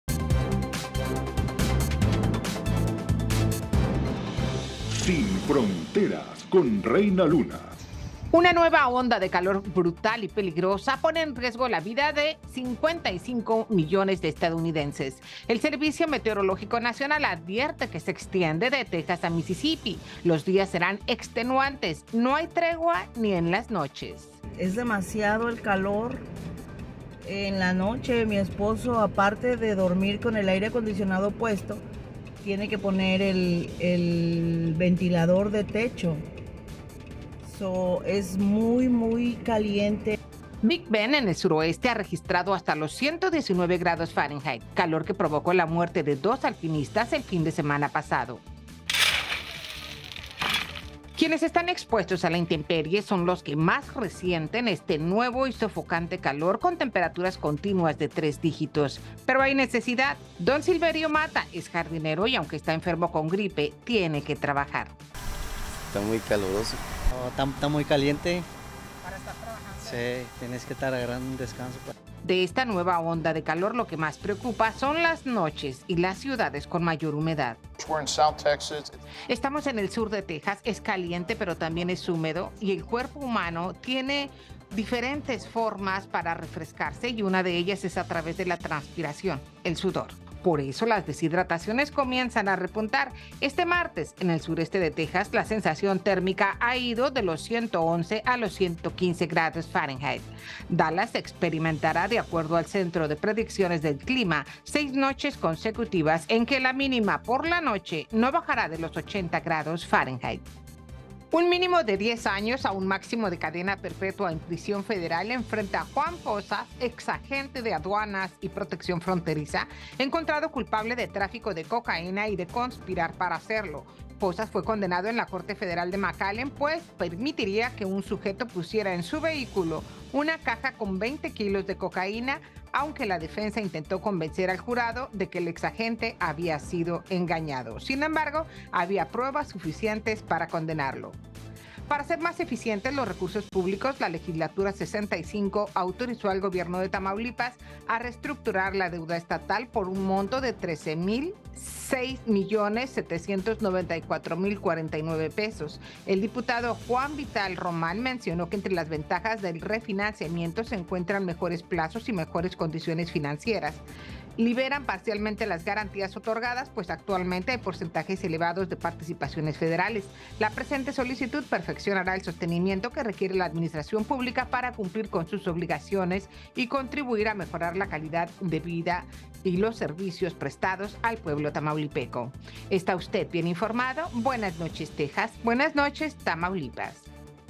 ¡¡¡Se avecina la madre de todos los calores… millones de vidas en peligro!!!
Agente federal “chueco” lo condenan por traficar coca… Autorizan reestructurar deuda de Tamaulipas.